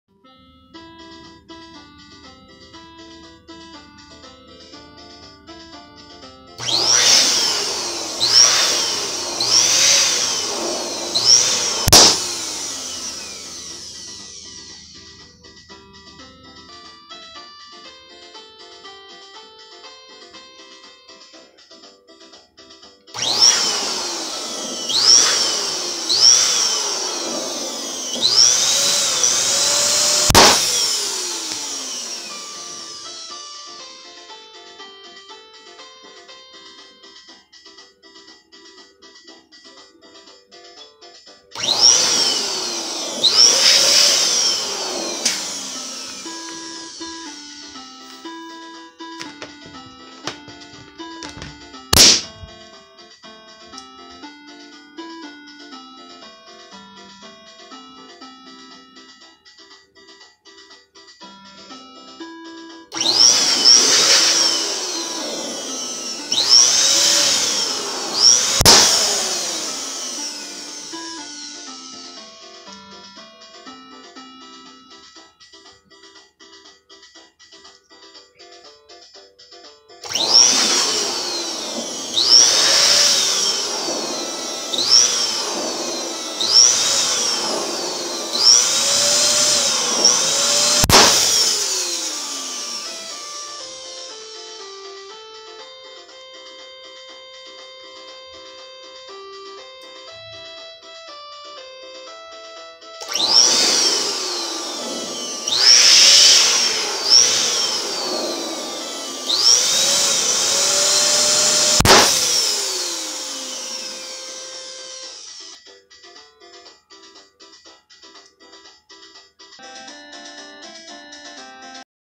SO IT'S YOUR BIRTHDAY!! LETS PUMP TO POP BIRTHDAY BALLOONS!
Let's pop Happy Birthday Balloons!

Welcome to Vipi Balloon Show!